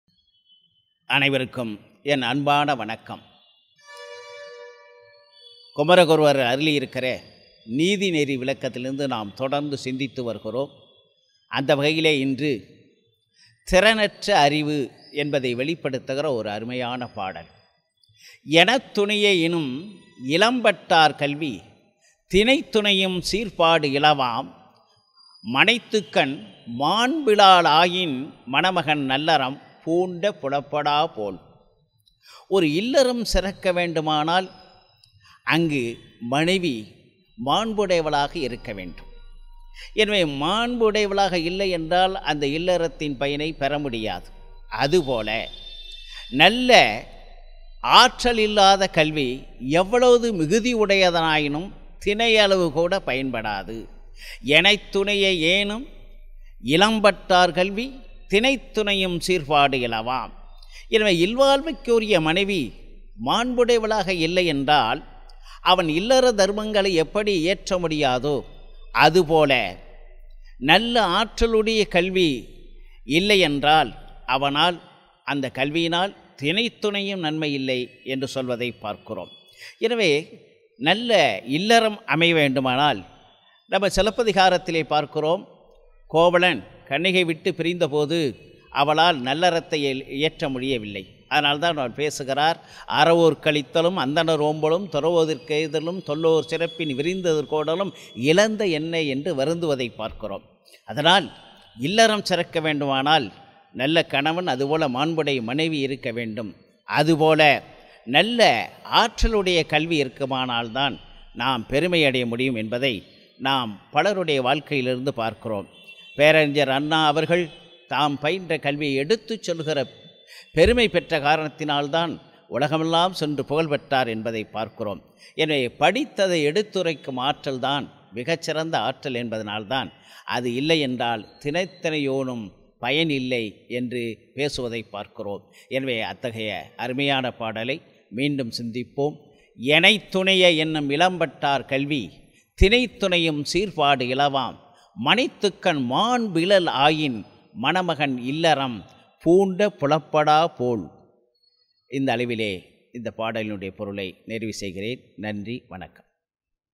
திறனற்ற அறிவு, Neethi Neri Vilakkam - 9, நீதிநெறி விளக்கம், எனைத்துணைய வேனு மிலம், கருணா சேகர்
எனைத்துணைய வேனு மிலம்பாட்டார் கல்வி
தினைத்துணையுஞ் சீர்ப்பா டிலவாம் - மனைத்தக்காள்
பாண்பில ளாயின் மணமக னல்லறம்
பூண்ட புலப்படா போல்-9